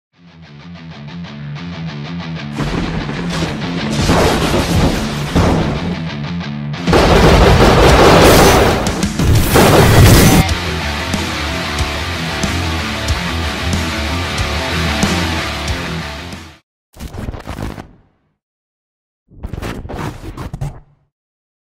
World Record Bench Press
World record equipped bench press of 1105 lbs by Will Barotti